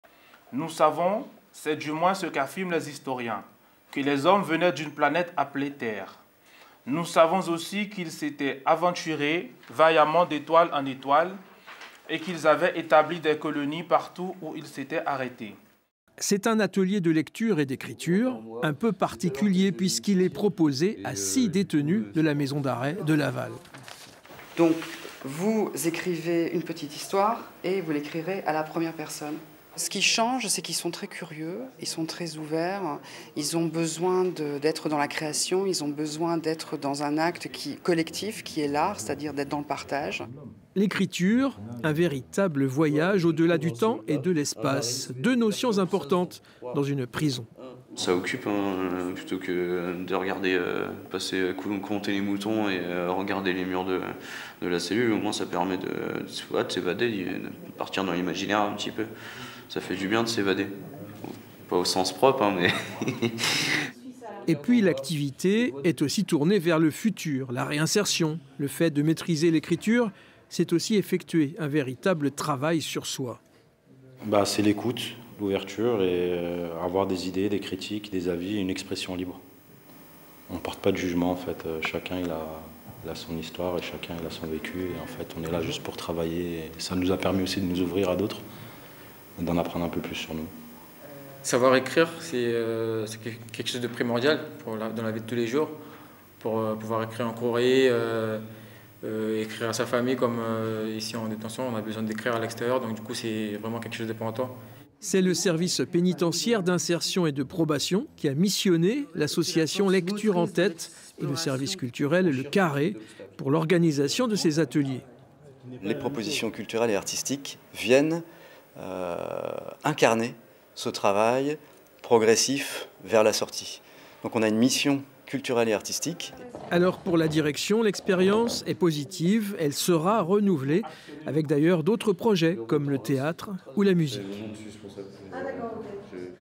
Écrire pour échapper à la prison
Ecrire pour échapper à la prison le temps d'un atelier, c'est l'objectif de l'association Lecture en tête qui organise dans la maison d'arrêt de Laval des rencontres entre écrivains et détenus.
Il y a quelques jours, c'est le journaliste et auteur Denis Michelis qui s'est rendu sur place.
Reportage Pierre-Erik Cally et Florie Cottenceau.